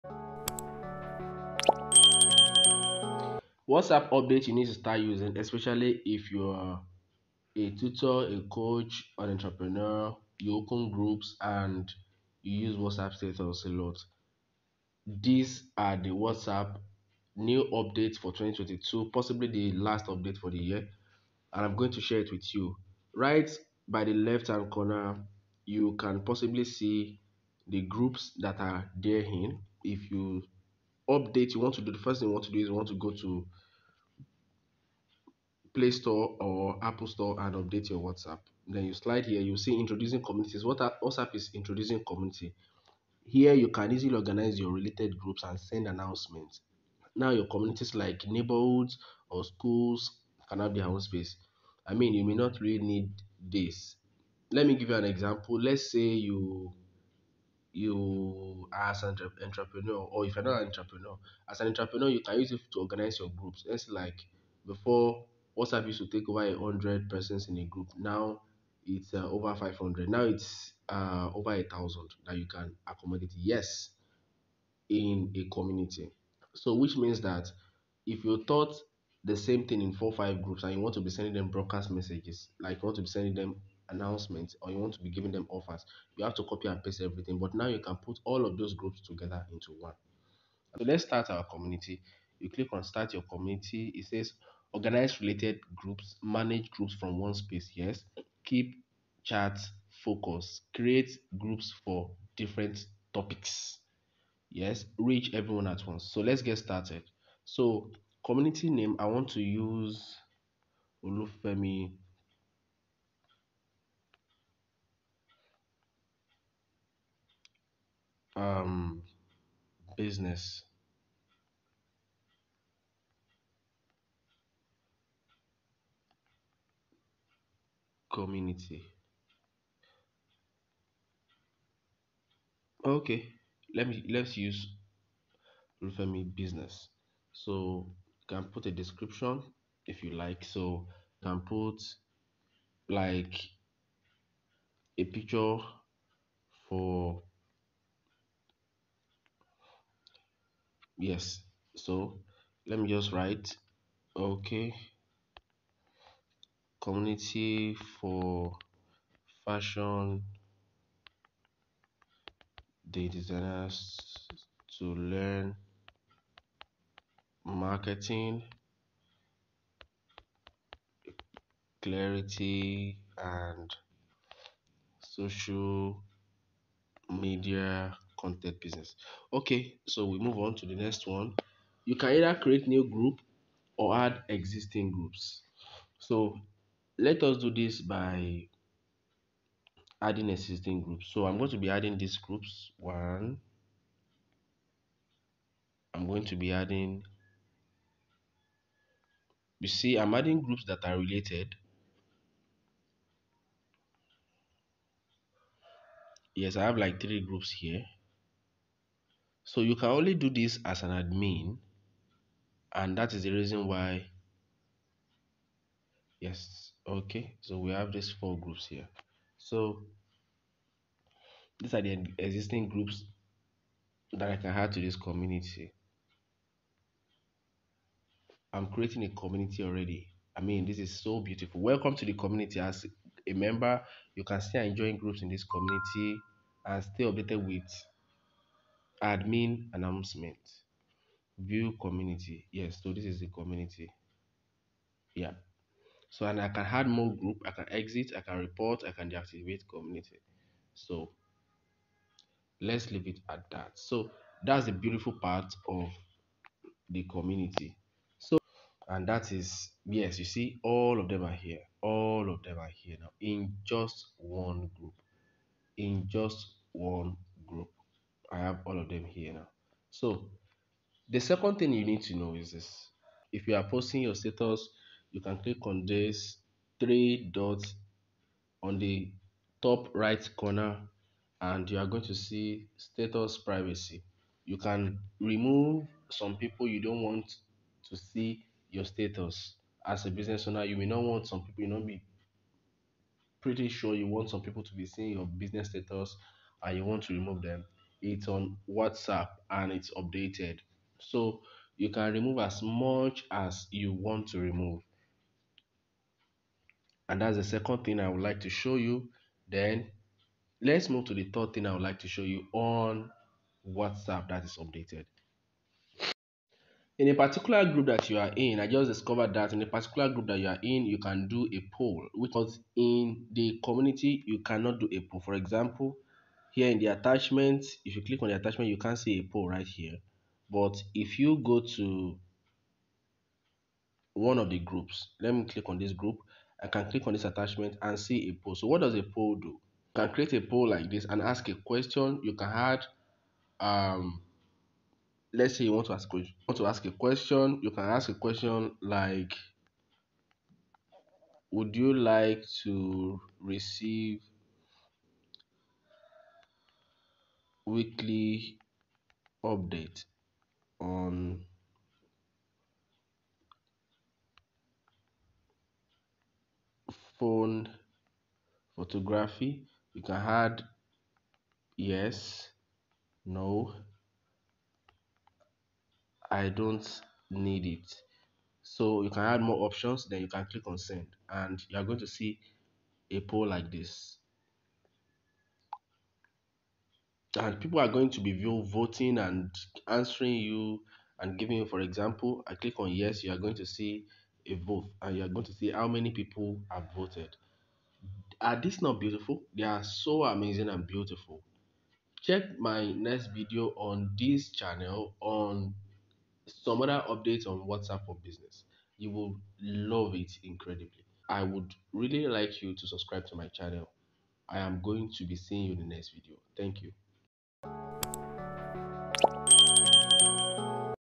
Whatsapp Update 2022 | WhatsApp Community | Poll
WhatsApp community helps you to group all your groups into one community so that you can make an announcement to them all at once.

watch this video as I explain in clear detail how you can do that and also create polls that can help you to get feedback from your customers.